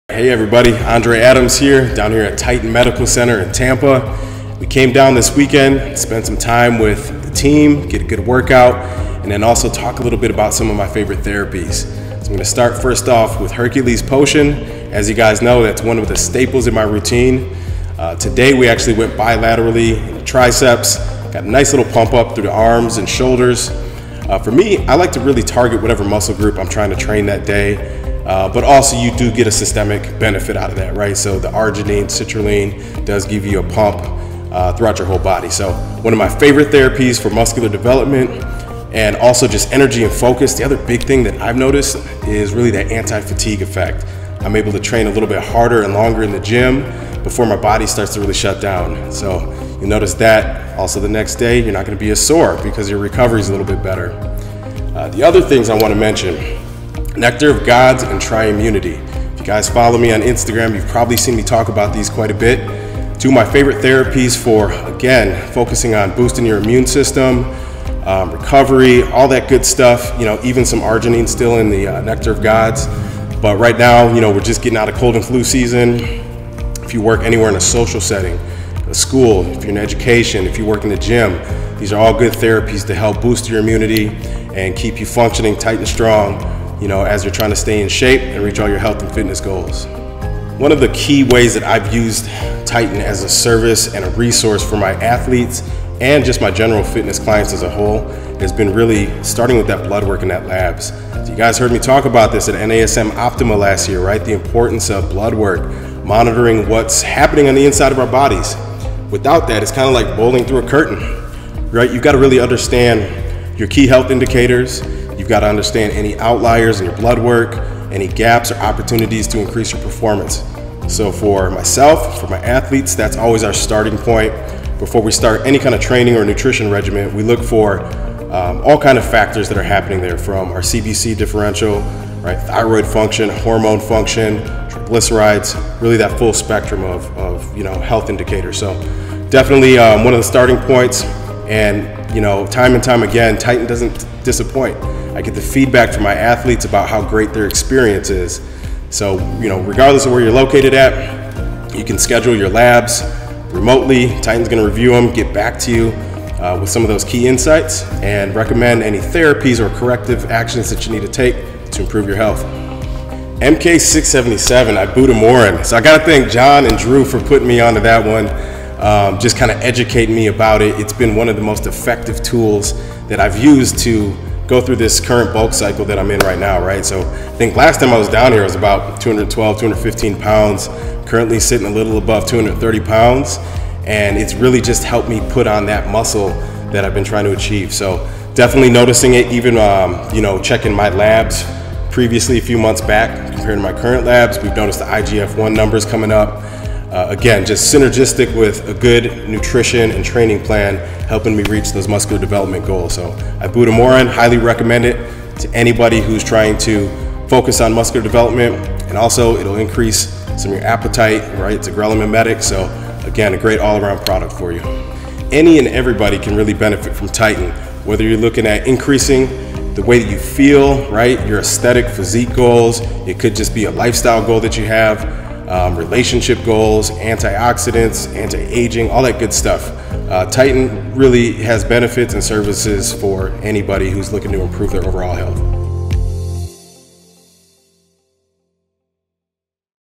IFBB Pro Olympian & NASM Master Trainer Andre Adams Testimonial!
IFBB Pro Olympian & NASM Master Trainer Andre Adams talks about his favorite Titan Medical Center therapies and how Titan has helped him in his health & fitness journey!